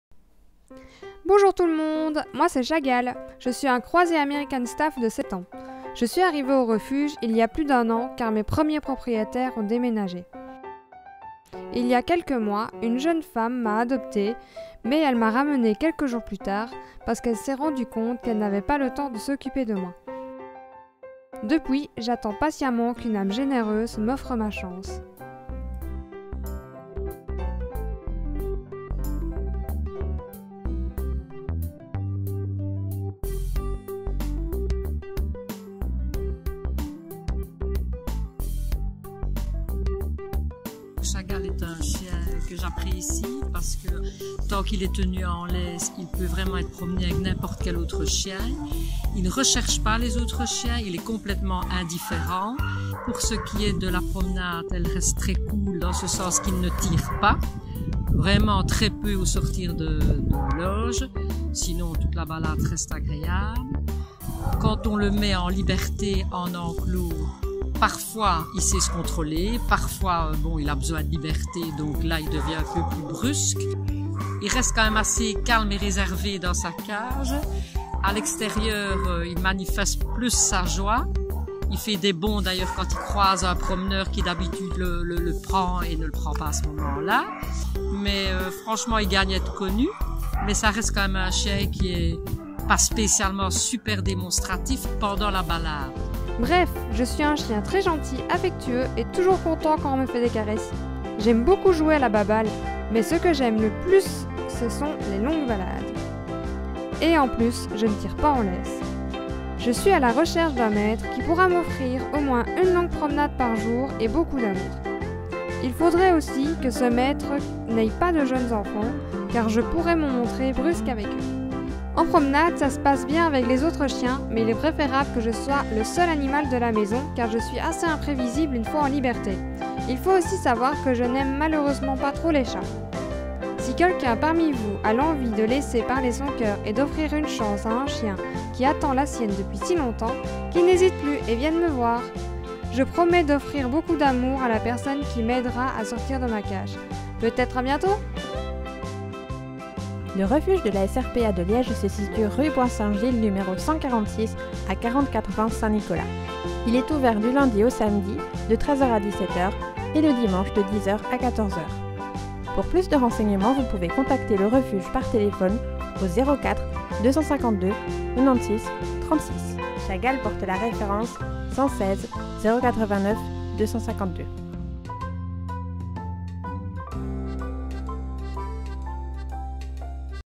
ADOPTÉ....... chagall chien à adopter
Je vais vous demander un élan de générosité pour un chien qui se trouve en cage depuis plus d'un an à la SRPA de Liège. Il s'agit de Chagall, croisé american staff de 7 ans. J'aimerais que chacun d'entre-vous partage cette vidéo afin de lui donner une chance supplémentaire de trouver une famille. Nous commençons sérieusement à désespérer et à penser qu'il ne trouvera jamais sa famille. Le temps passe si vite. Alors, je me dis que si chaque personne partage, nous aurons peut-être la chance d'attirer l'attention de quelqu'un et d'offrir à Chagall sa chance. Je souhaiterais que nous commencions la plus grande chaîne de solidarité pour ce chien qui ne mérite pas une vie pareille. Merci à celles et ceux qui le feront.
Vous trouverez plus de renseignements sur Darius sur le site de la SRPA sur le facebook officiel de la SRPA ou tout simplement en téléphonant à un employé de l'accueil. Le refuge est ouvert tous les jours de 13h à 17h et le dimanche de 10h à 14h.